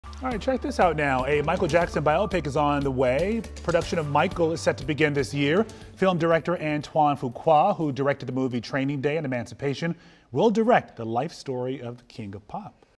Michael Jackson biopic is on the way
A Michael Jackson Bio-pic is on the way. Production of "Michael" is set to begin this year.